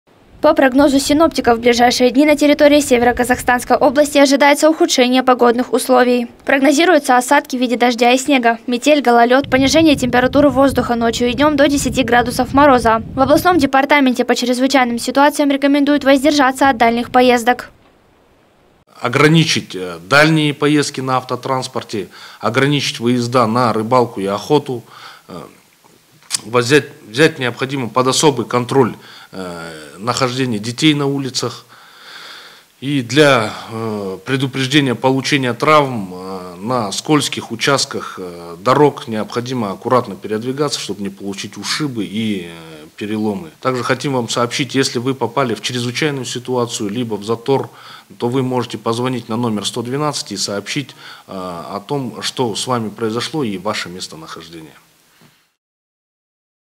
Шторомовое предупреждение на территории Северо-Казахстанской области
По прогнозу синоптиков в ближайшие дни на территории Северо-Казахстанской области ожидается ухудшение погодных условий.